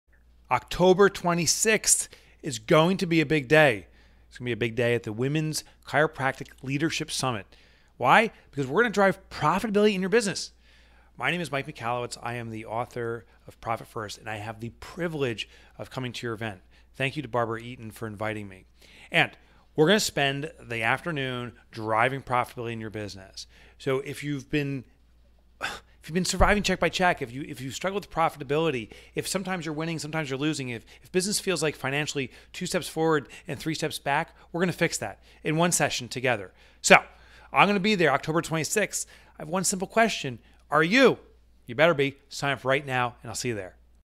Mike Michalowicz Seminar will be a LIFE University!
Mike Michalowicz joins me for The Women's Chiropractic Leadership Summit 2019.

Click here for to join us!